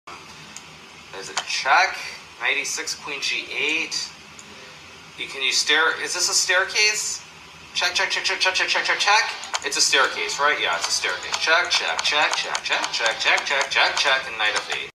Ever seen a STAIRCASE CHECK before? watch this video by HIKARU & Subscribe
Hikaru plays the staircase check, very brilliant and fun to watch video